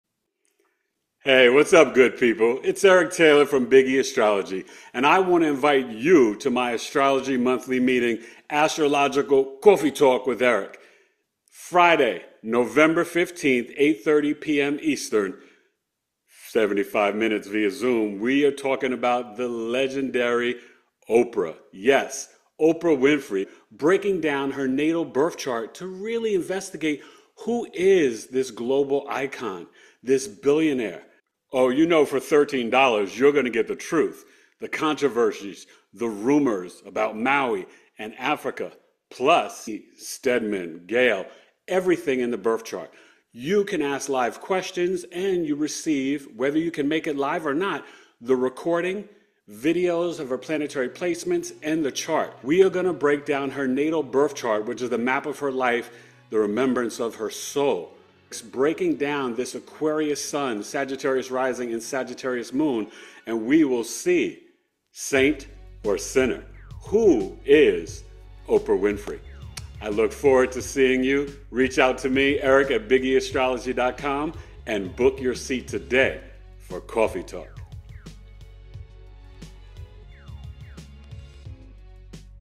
Astrological Coffee Talk e/ Eric Fri Nov 15th, 8:30 PM EST Oprah Winfrey Saint or Sinner
Big e Astrology- I am inviting you to my Special Mini-Coffee Talk Event on Friday, Nov 15th at 8:30 PM EST for 75 minutes via Zoom for $13 looking at the Natal Birth Chart of Oprah Winfrey Saint or Sinner. We will look at every aspect of her life including Gayle, Stedman, O Magazine, Oprah Winfrey Show, OWN Network, Maui & Africa School controversies, and more. If you can't make it live you can still send in the payment and receive the recording, videos, chart & more.